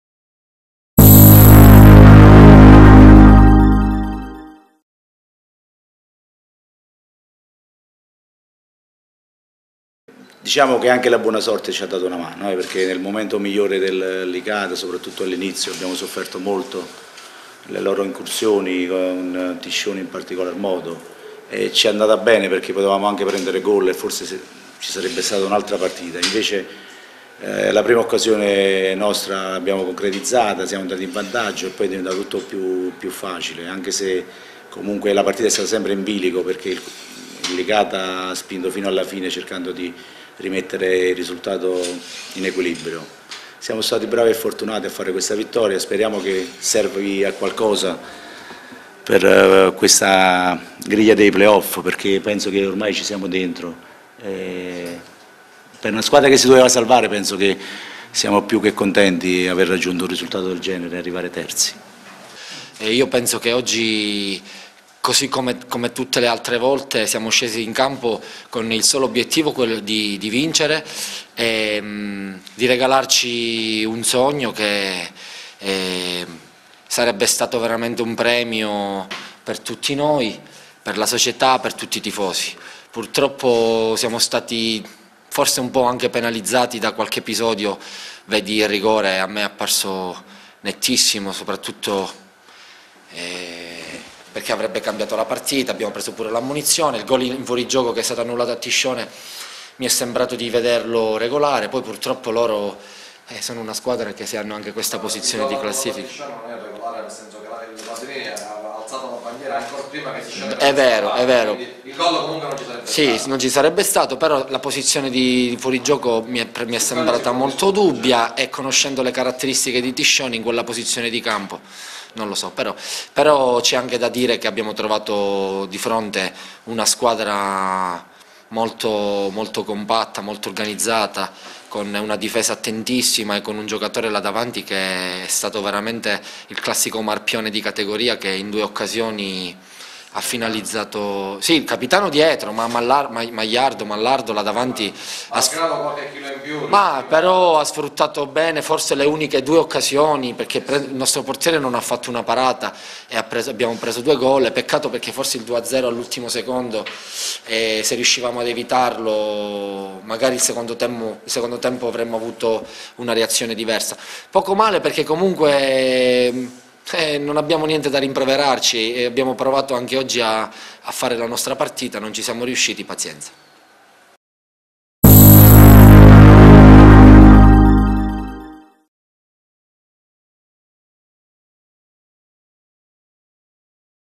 Le interviste di Licata-Battipagliese
Il tecnico della Battipagliese Andrea Ciaramella e il centrocampista del Licata Rosario La Marca intervistati nel dopo gara